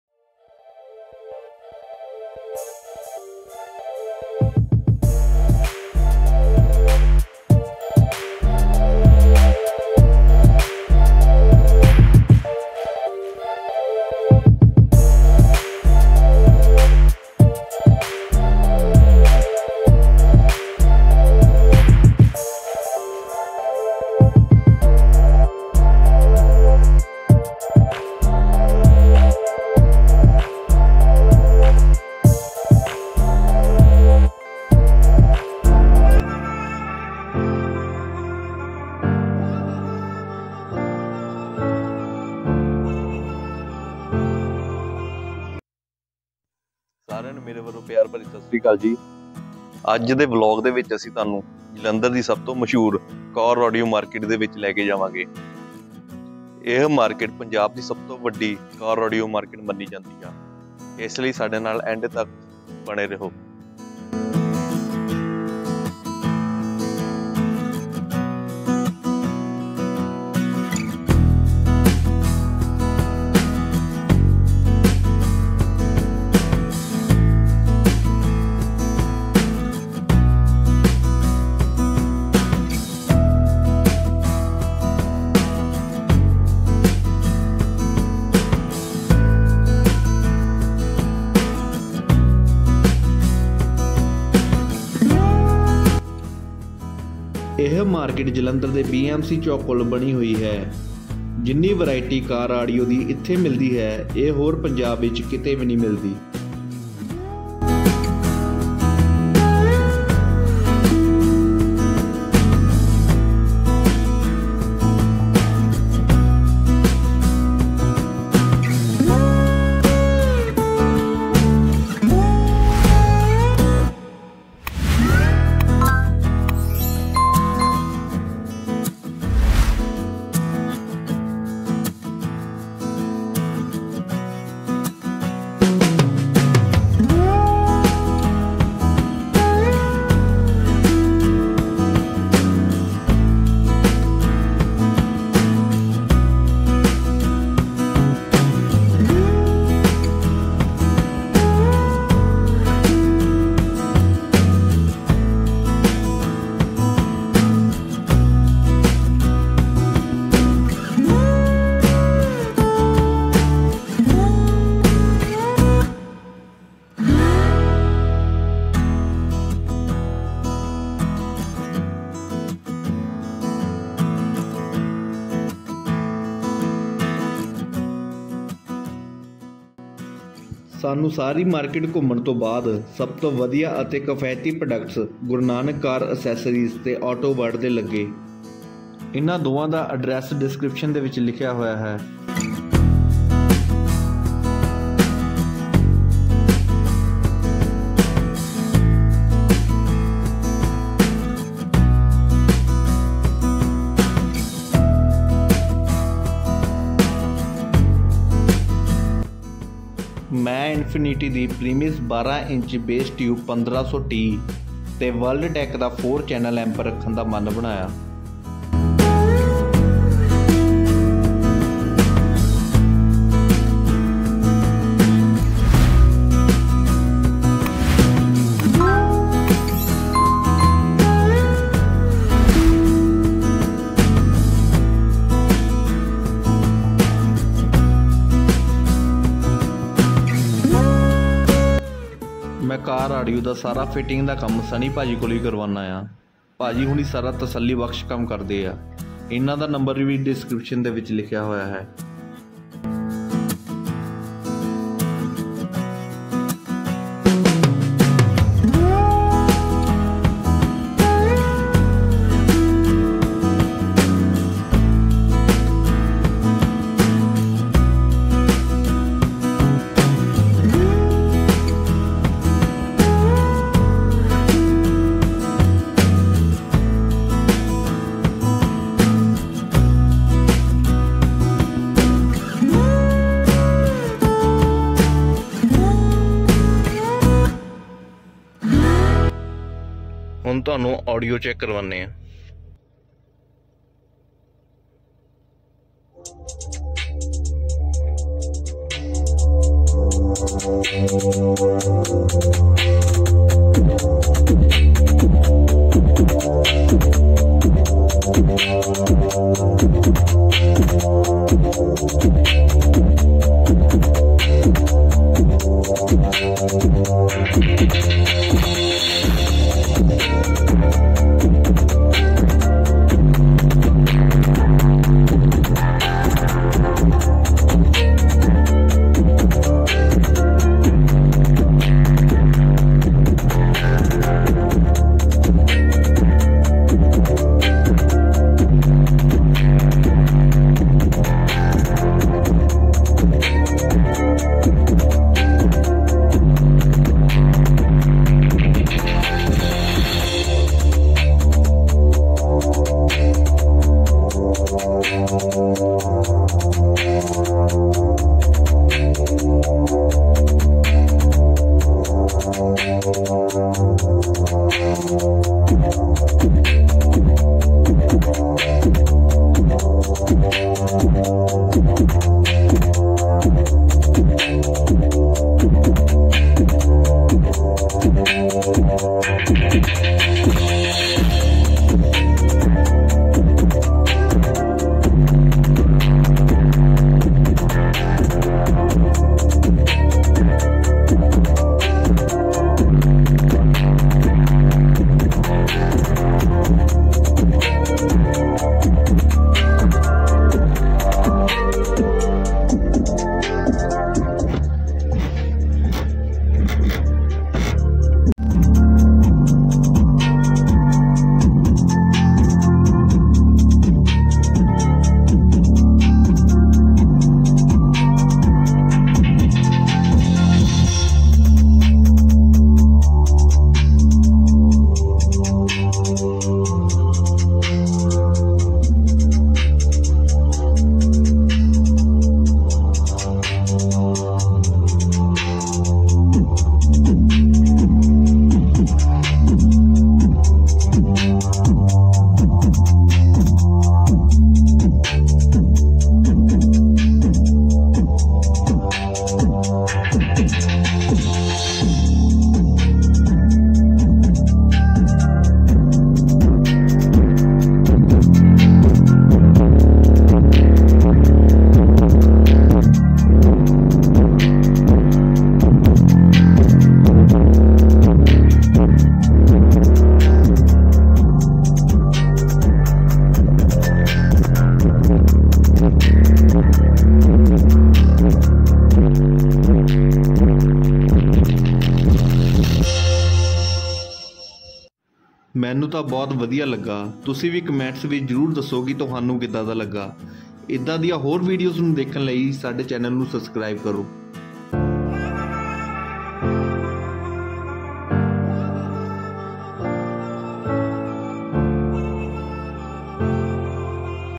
Punjab's Most Famous Car Audio Market|ਪੰਜਾਬ ਦੀ ਸੱਭ ਤੋਂ ਮਸ਼ਹੂਰ ਕਾਰ ਆਡੀਓ ਮਾਰਕੀਟ|INFINITY 1500TBassTube
Punjab's Most Famous Car Audio Market|ਪੰਜਾਬ ਦੀ ਸੱਭ ਤੋਂ ਮਸ਼ਹੂਰ ਕਾਰ ਆਡੀਓ ਮਾਰਕੀਟ

INFINITY Subpr 1500T Primus 12 inch Tube Woofer Subwoofer (Passive, RMS Power: 325 W) | World tech 4 Channel Amplifier

Auto World
8, Tehsil Rd, New Court Chowk, Jawahar Nagar, Jalandhar, Punjab 144001.

Abhi Bhagat (Sunny)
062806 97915